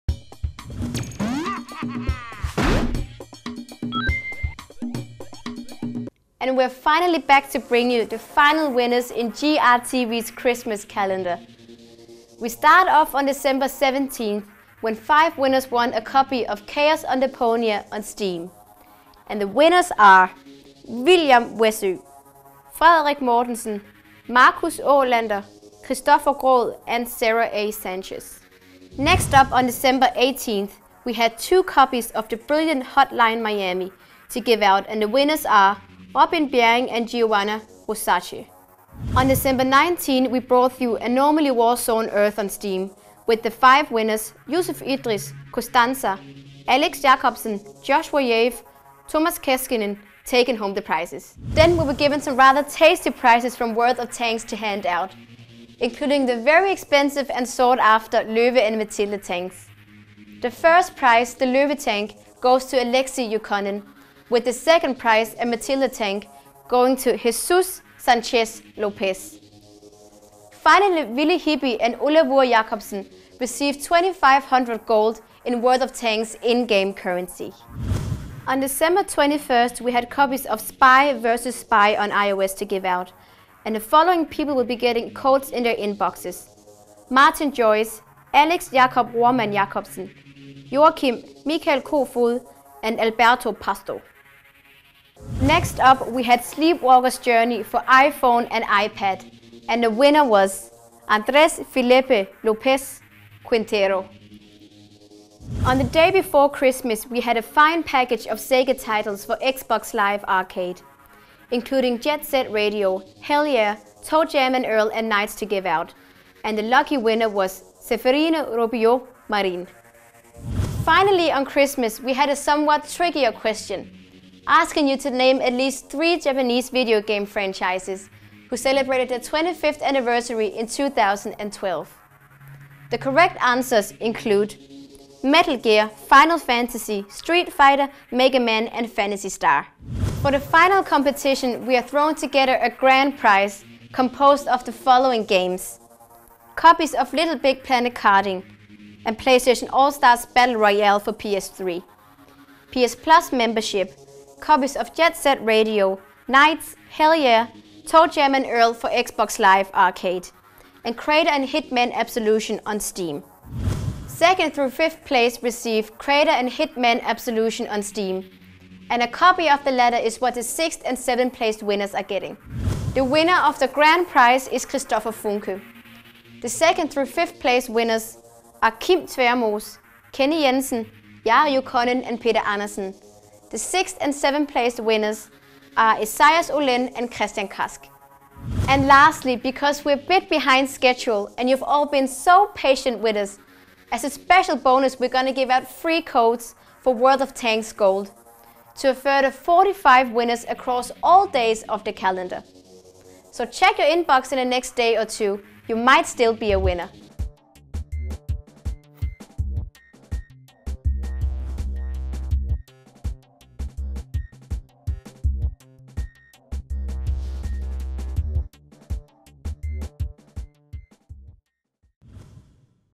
GRTV Xmas Calendar: Winners of Day 17th-24th
We picked out the last winners in our Xmas calendar compititions. Are you one of the lucky ones?